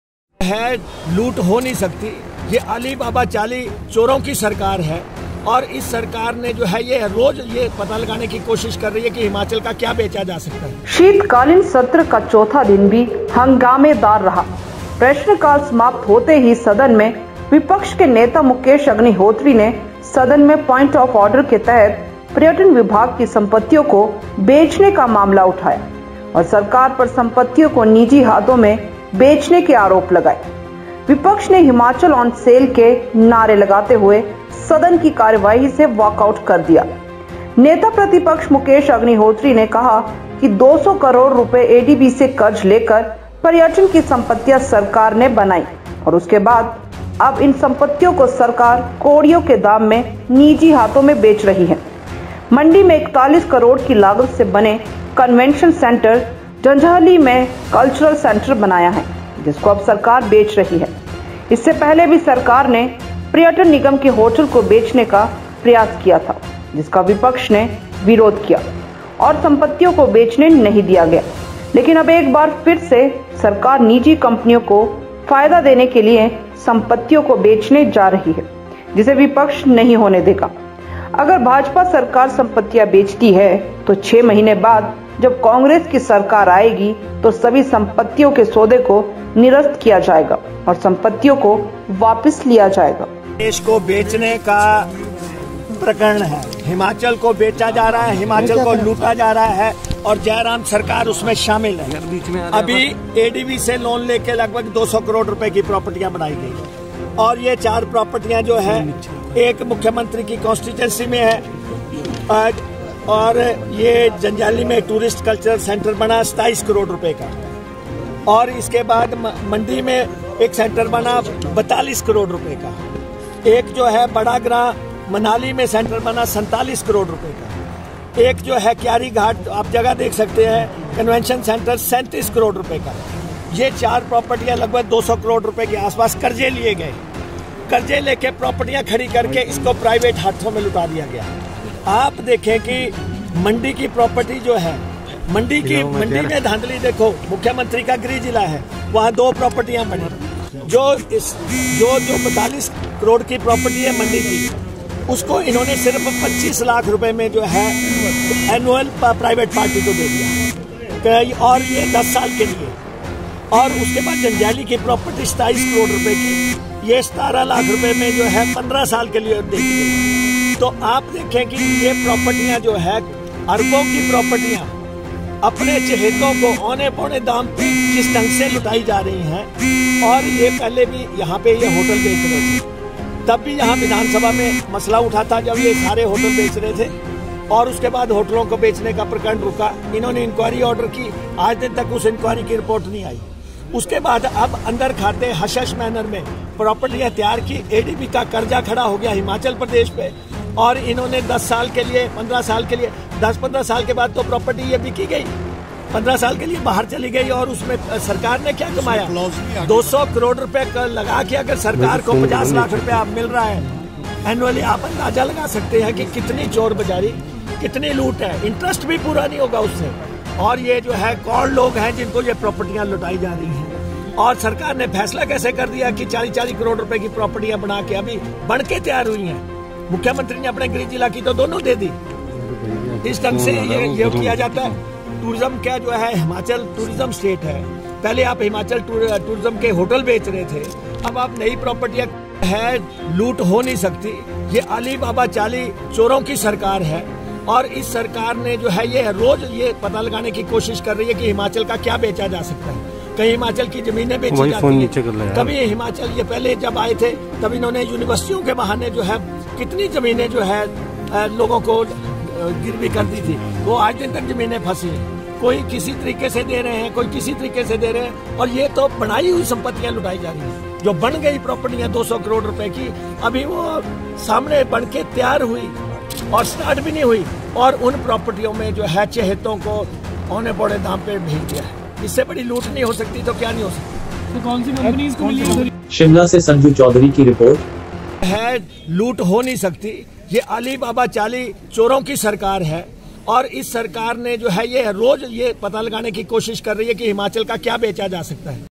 अलीबाबा चालीस चोर की सरकार है हिमाचल की भाजपा सरकार : मुकेश अग्निहोत्री
Alibaba is a government of forty thieves, the BJP government of Himachal: Mukesh Agnihotri